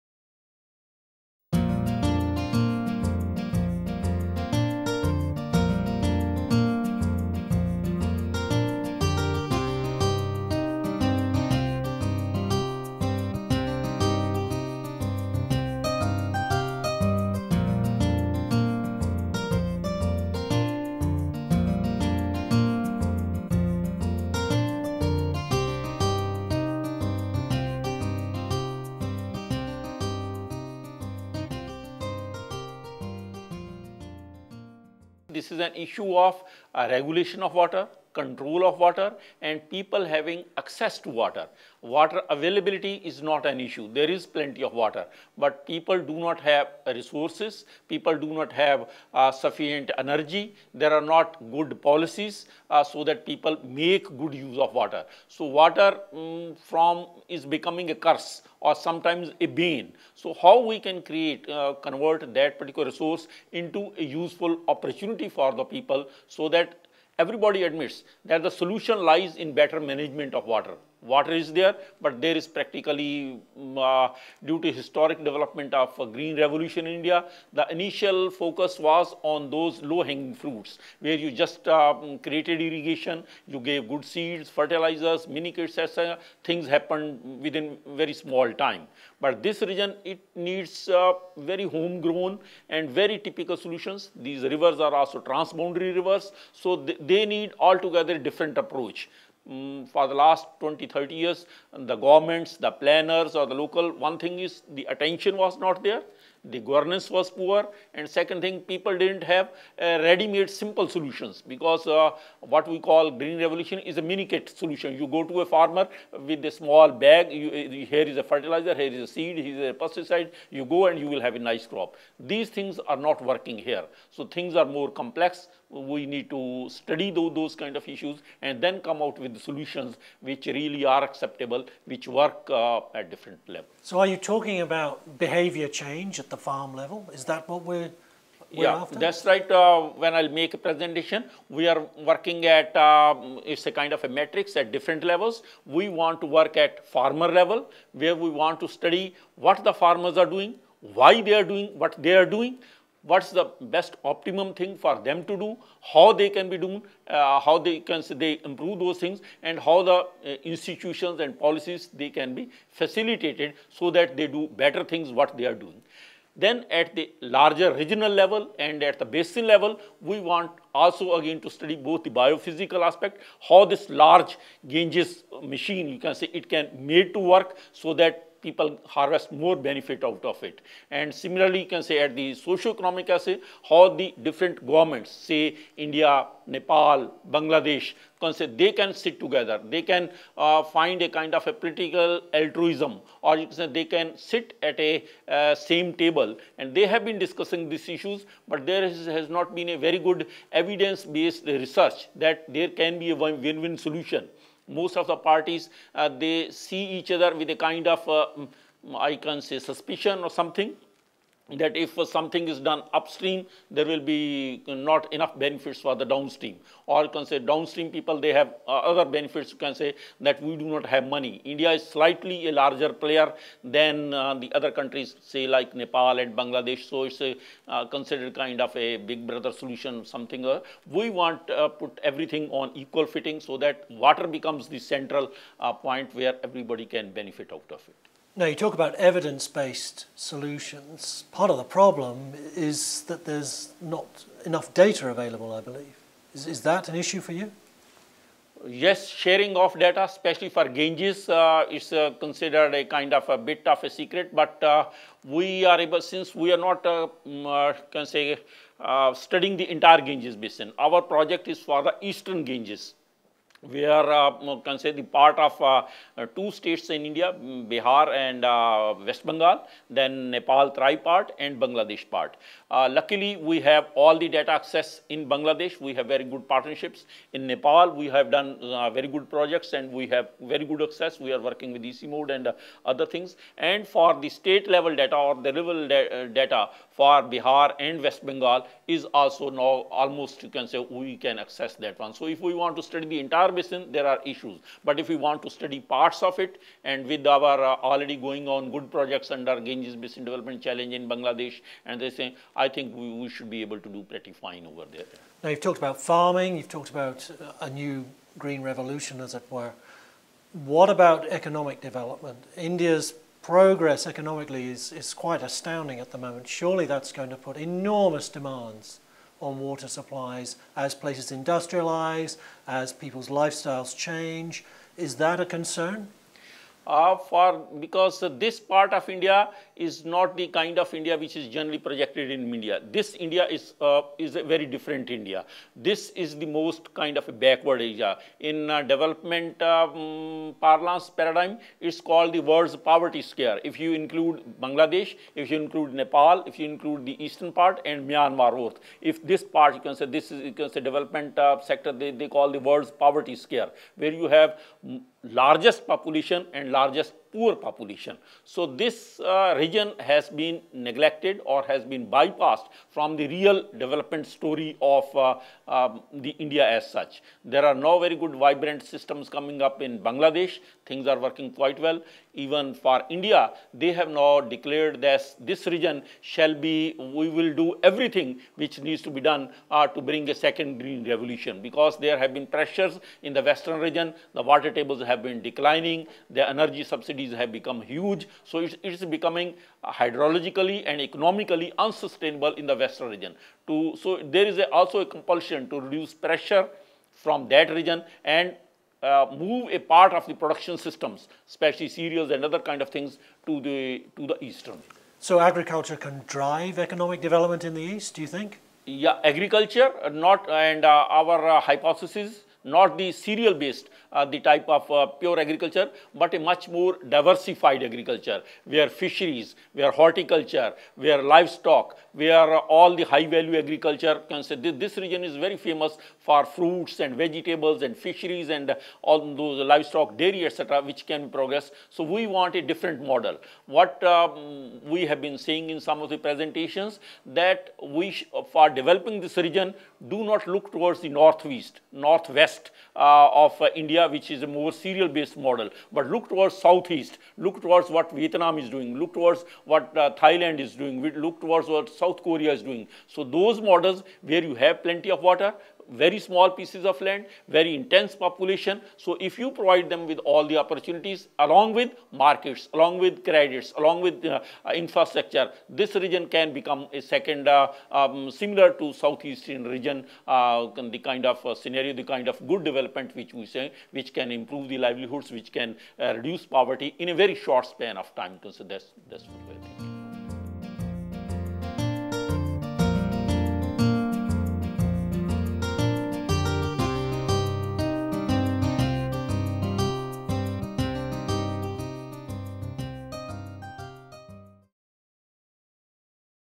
Turning India's eastern Gangetic plain into a productive agricultural powerhouse - Bharat Sharma
Water management is the key that can turn India's Eastern Gangetic Plain into a productive agricultural powerhouse, argues IWMI's Bharat Sharma.